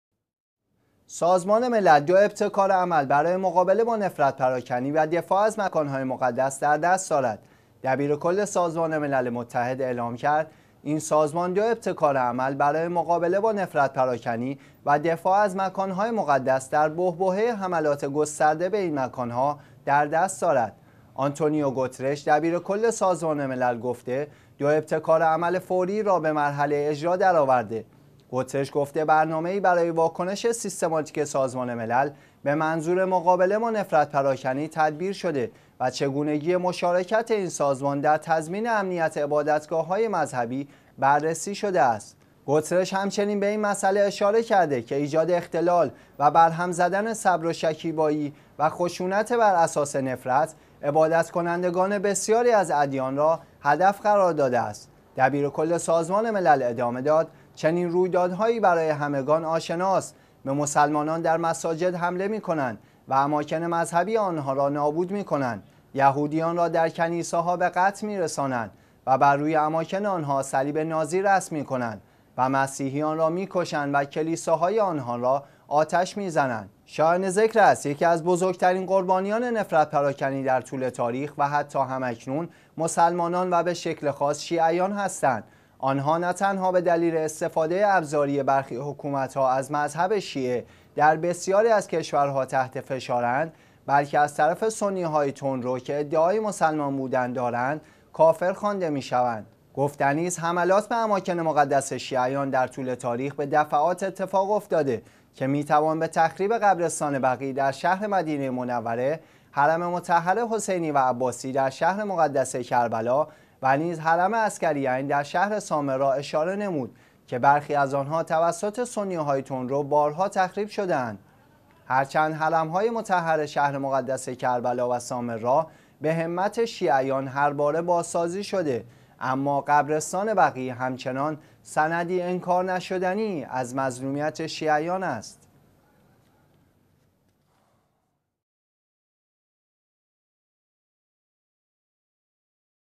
تلاش سازمان ملل متحد برای دفاع از مکان‌های مقدس
سازمان ملل دو ابتکار عمل برای مقابله با نفرت‌پراکنی و دفاع از مکان‌های مقدس در دست دارد.
دبیرکل سازمان ملل متحد اعلام کرد: این سازمان دو ابتکار عمل برای مقابله با نفرت‌پراکنی و دفاع از مکان‌های مقدس در بحبوحه حملات گسترده به این مکان‌ها در دست دارد.
«آنتونیو گوترش» دبیرکل سازمان ملل گفته دو ابتکار عمل فوری را به مرحله اجرا درآورده‌؛
گوترش گفته برنامه‌ای برای واکنش سیستماتیک سازمان ملل به منظور مقابله با نفرت پراکنی تدبیر شده و چگونگی مشارکت این سازمان در تضمین امنیت عبادتگاه‌های مذهبی بررسی شده است
گوترش همچنین به این مساله نیز اشاره کرده که ایجاد اختلال و بر هم زدن صبر و شکیبایی و خشونتِ بر اساس نفرت، عبادت کنندگانِ بسیاری از ادیان را هدف قرار داده است.
دبیرکل سازمان ملل ادامه داد: چنین رویدادهایی برای همگان آشنا است؛به مسلمانان در مساجد حمله می‌کنند و اماکن مذهبی آنها را نابود می‌کنند؛ یهودیان را در کنیسه‌ها به قتل می‌رسانند و بر روی اماکن آنها صلیب نازی رسم می‌کنند و مسیحیان را می‌کشند و کلیساهای آنها را آتش می‌زنند.
شایان ذکر است، یکی از بزرگترین قربانیان نفرت پراکنی در طول تاریخ و حتی هم اکنون مسلمانان و به شکل خاص شیعیان هستند؛ آنها نه تنها به دلیل استفاده ابزاری برخی حکومت ها از مذهب شیعه، در بسیاری از کشورها تحت فشارند، بلکه از طرف ، سنی های تندرو که ادعای مسلمان بودند دارند، کافر خوانده می شوند.
گفتنی است حملات به اماکن مقدس شیعیان در طول تاریخ به دفعات اتفاق افتاده که می توان به تخریب قبرستان بقیع در شهر مدینه منوره، حرم مطهر حسینی و عباسی در شهر مقدس کربلا و نیز حرم عسکریین در شهر سامرا اشاره نمود که برخی از آنها توسط سنی های تندرو بارها تخریب شده اند.
هر چند حرم های مطهر شهر مقدس کربلا و سامرا به همت شیعیان هر بار بازسازی شده، اما قبرستان بقیع همچنان سندی انکار نشدنی از مظلومیت شیعیان است.